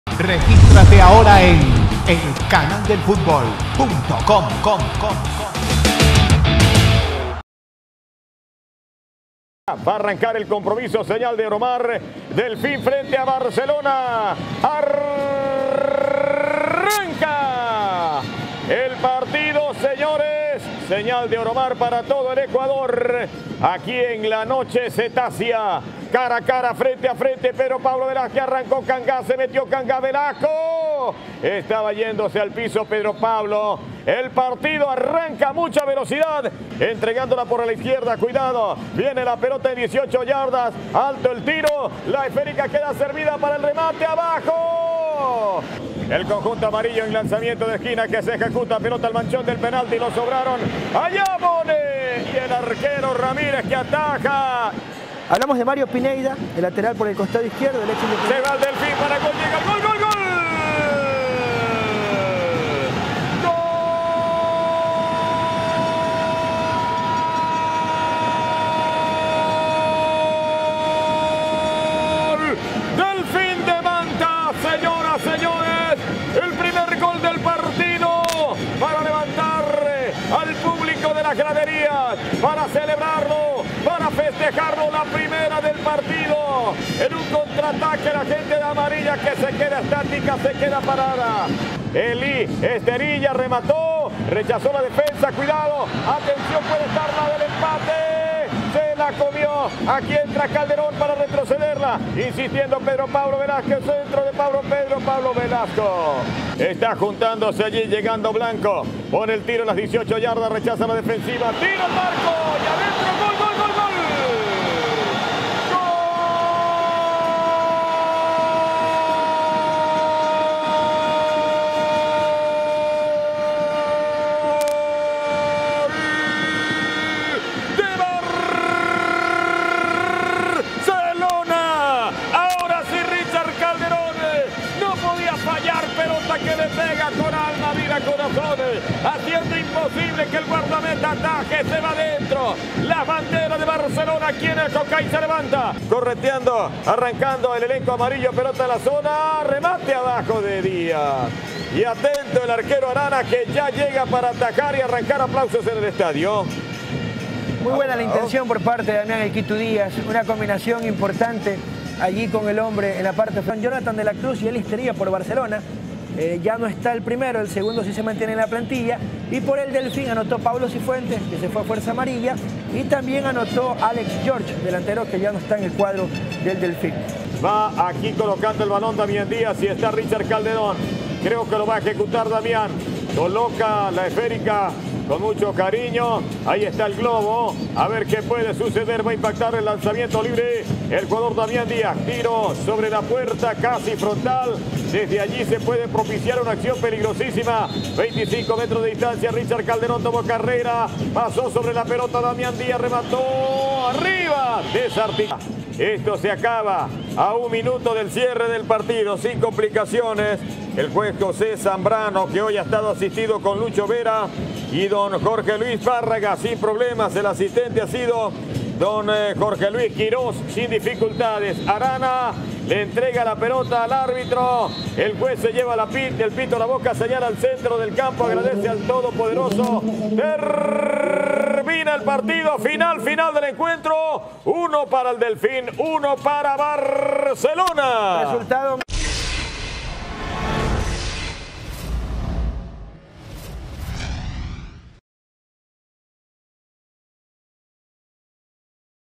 DELFIN 1 BARCELONA 1 NOCHE CETÁCEA 2016
El cuadro cetáceo empató a un gol con los toreros en la presentación de la plantilla del equipo manabita.